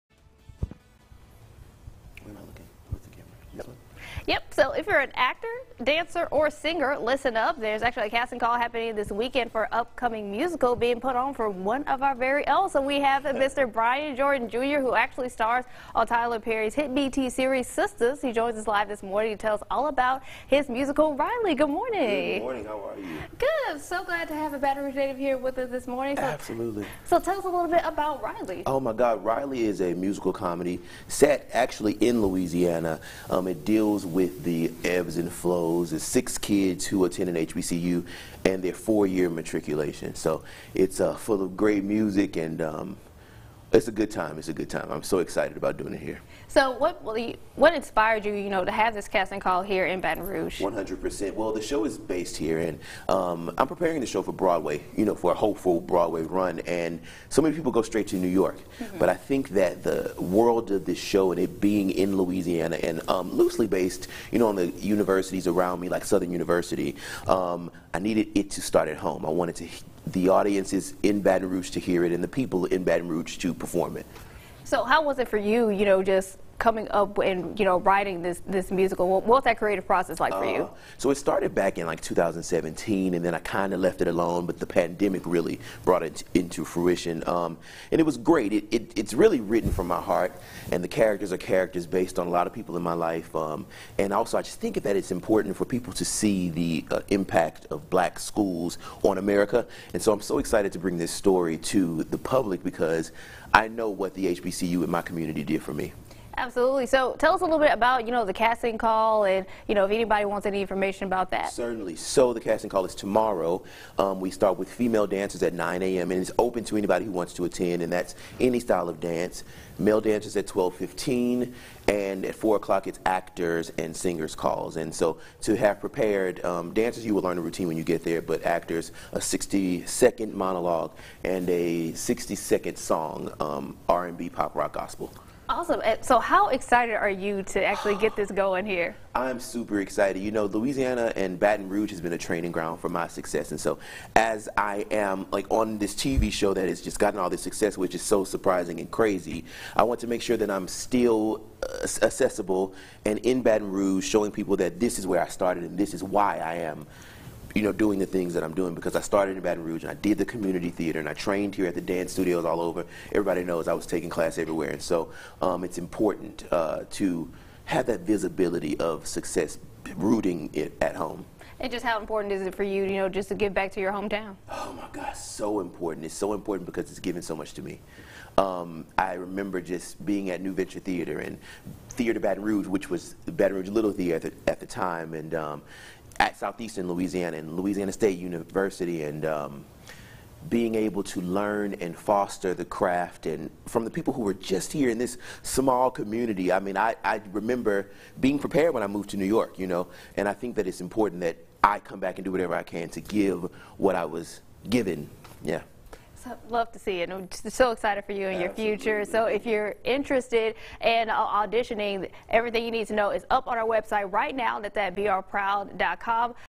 Baton Rouge actor talks about audition & inspiration for his new musical 'Riley'
Brian Jordan Jr. talks about 'Riley' musical inspired by his upbringing in Baton Rouge